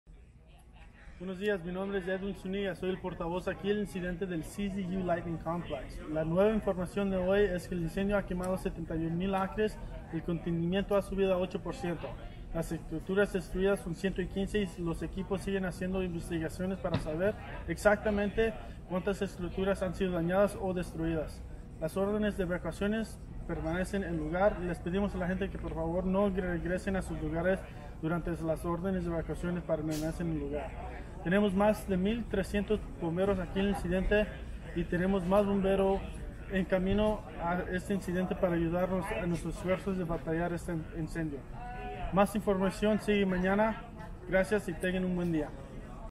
CZU Lightning Complex update (Spanish) Aug 23
Esta es una actualizacion importante del incendio